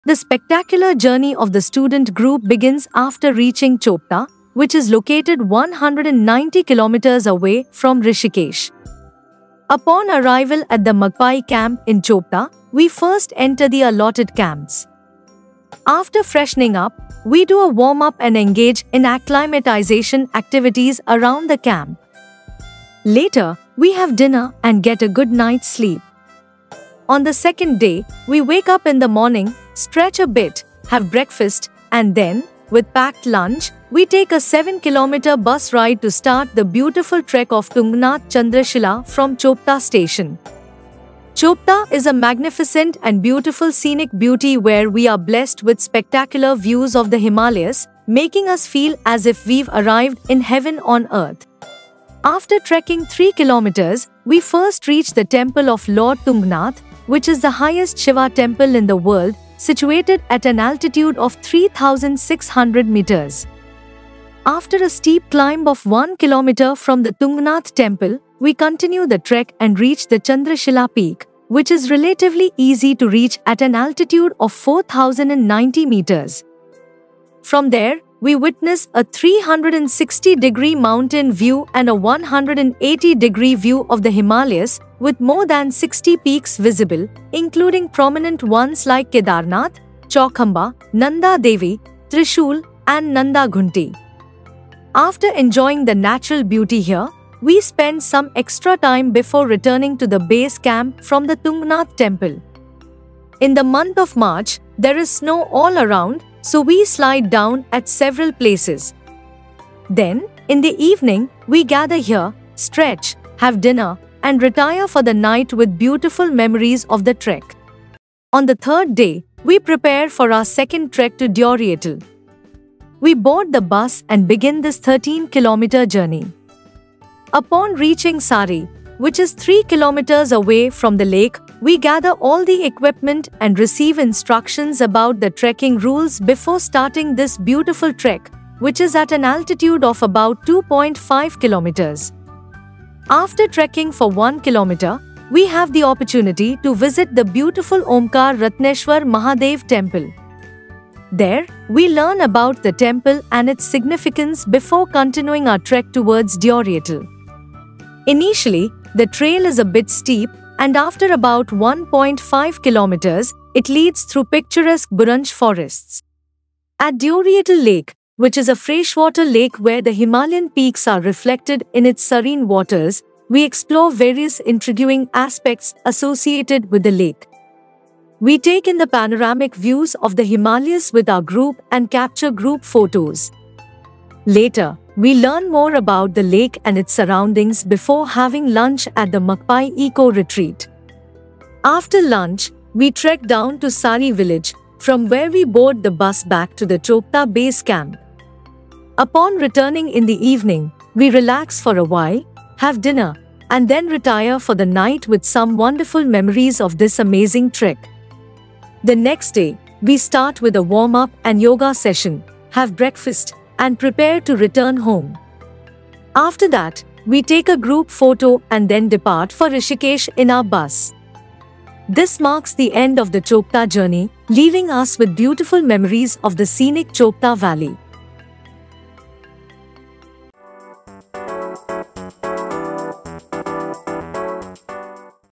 Student Group Tour | Chopta Uttarakhand | Chopta - Tungnath-Chandrashila | Deoria Tal
Join us on this unforgettable journey of exploration, adventure, and learning. Create lasting memories with your peers as you discover the natural wonders of Rishikesh and Chopta. Magpie Group invites student groups to embark on this transformative experience and unlock the beauty of Uttarakhand.

Join the adventure today and let the mountains be your playground!